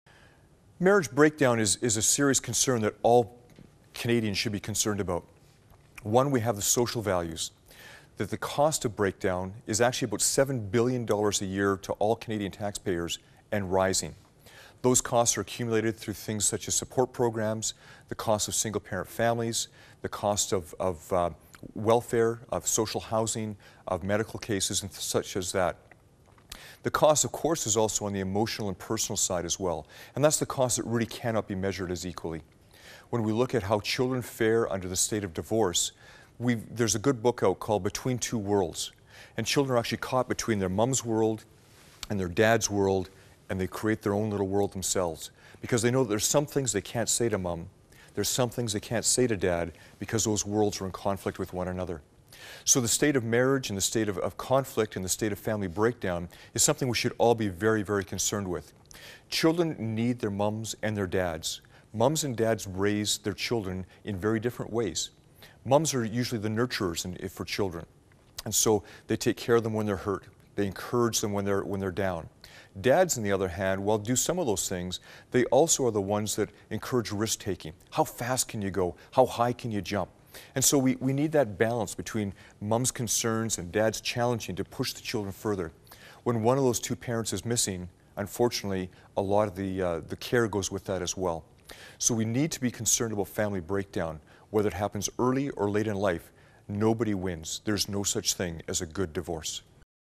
Web Exclusive!--"How prevalent is marriage breakdown in Canadian families today?"--Dave Quist
Dave Quist is the Executive Director of the Institute of Marriage and Family Canada. We asked him some questions many Canadian families are thinking about...